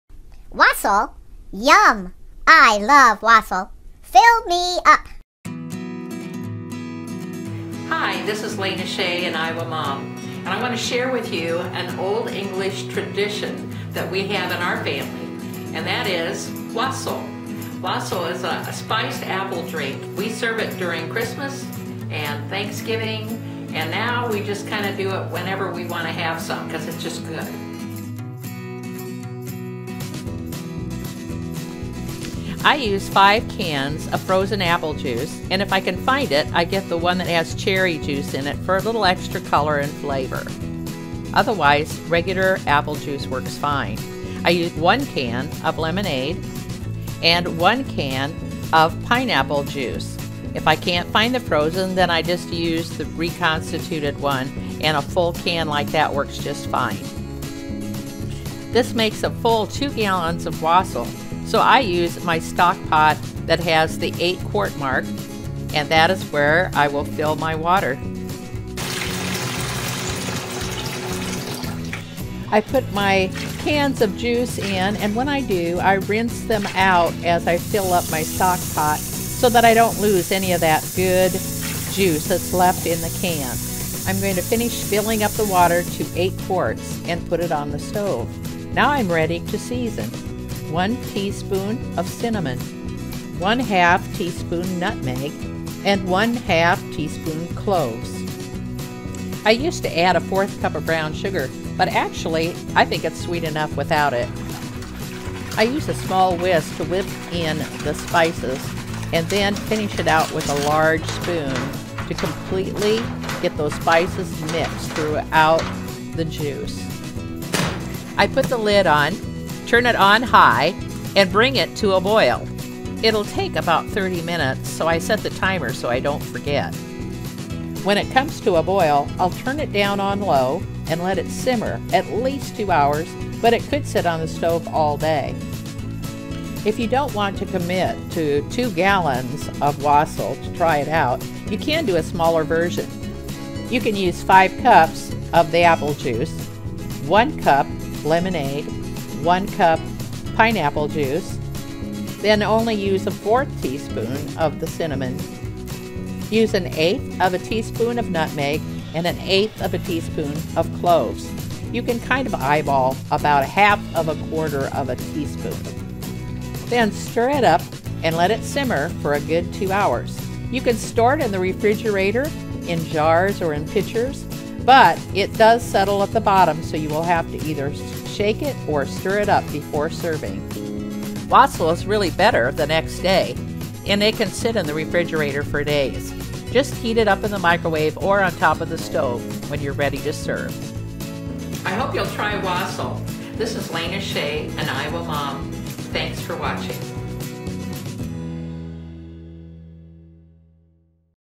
An Iowa Mom Makes Wassail
Wassail, an English spiced apple drink is traditionally served at Christmas but An Iowa Mom’s family keeps it in the frig to enjoy any time of year.

Wassail
As posted by Lana Shea on An Iowa Mom Videos (makes 2 gallons)

Reconstitute each can of juice according to directions on cans
4-5 cans frozen apple juice (or apple cherry juice for red tint)
1 can frozen lemonade
1 can frozen pineapple juice (or large can that doesn’t need reconstituting)
¼ cup brown sugar (optional)
1 tsp cinnamon
½ tsp cloves
½ tsp nutmeg

Simmer at least an hour.  All day is fine.  The flavor is even better after it sets for a day or two in the refrigerator.  Keeps well in the refrigerator.